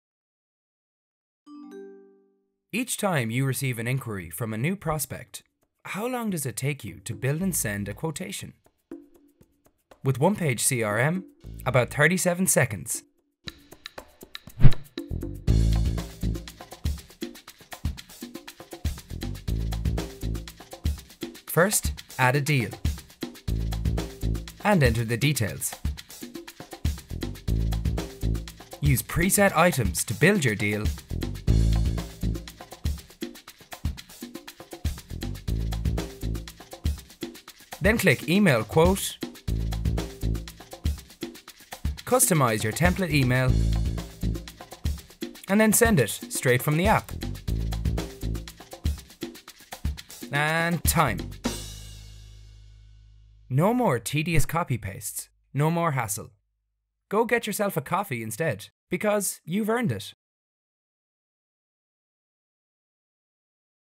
From Inquiry to Quote in 37 Seconds
OnePageCRM lets you build and email accurate sales quotes in just a few clicks, all without leaving the app.